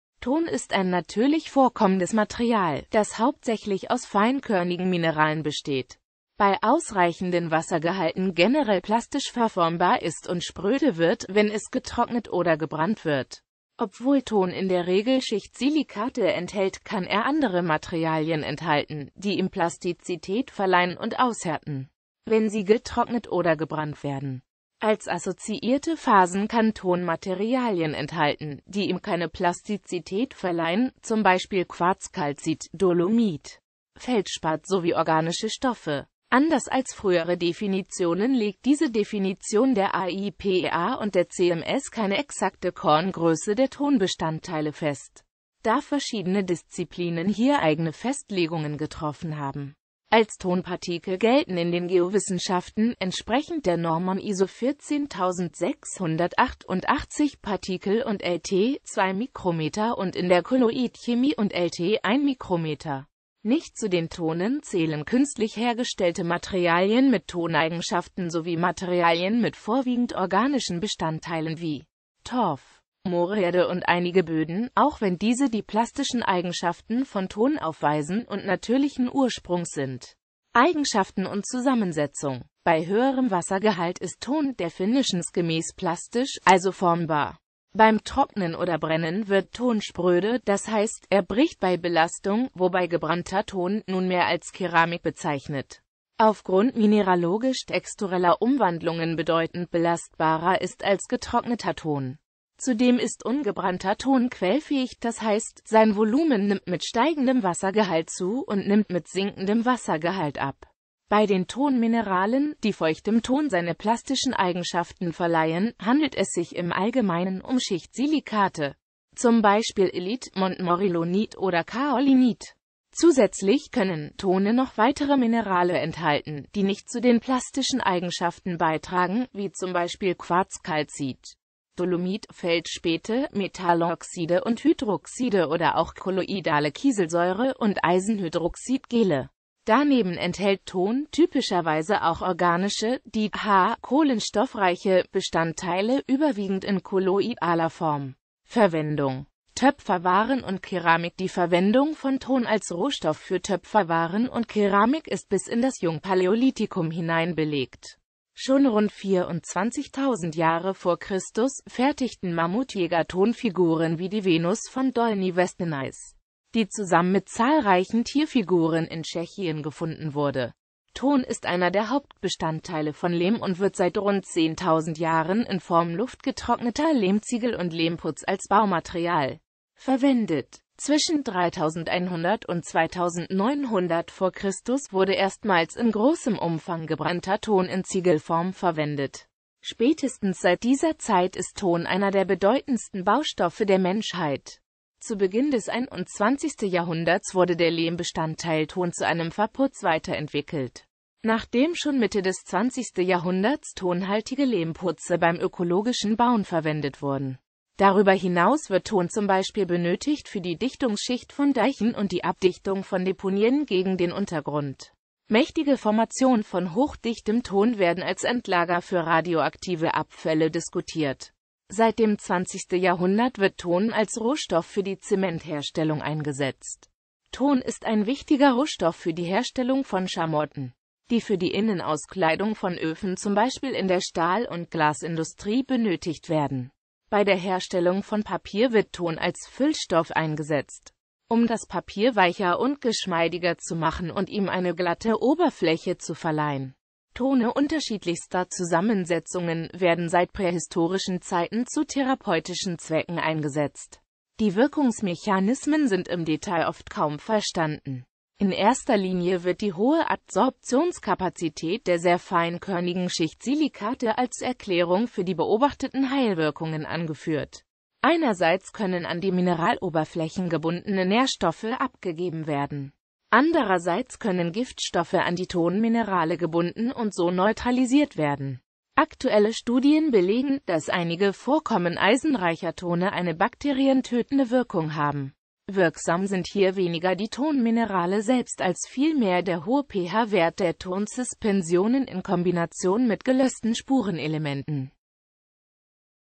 Ton (Bodenart)
Vielen Dank für Ihre Unterstützung:

Ton ist ein natürlich vorkommendes Material, das hauptsächlich aus feinkörnigen Mineralen besteht, bei ausreichenden Wassergehalten generell plastisch verformbar ist und spröde wird, wenn es getrocknet oder gebrannt wird.Obwohl Ton in der Regel Schichtsilikate enthält, kann er andere Materialien enthalten, die ihm Plastizität verleihen und aushärten, wenn sie getrocknet oder gebrannt werden.Als assoziierte Phasen kann Ton Materialien enthalten, die ihm keine Plastizität verleihen, zum Beispiel Quarz, Calcit, Dolomit, Feldspat sowie organische Stoffe.Anders als frühere Definitionen legt diese Definition der AIPEA und der CMS keine exakte Korngröße der Tonbestandteile fest, da verschiedene Disziplinen hier eigene Festlegungen getroffen haben.

Bild-Copyright-Informationen
Urheber Info: Die Autorenschaft wurde nicht in einer maschinell lesbaren Form angegeben. Es wird Siim als Autor angenommen (basierend auf den Rechteinhaber-Angaben).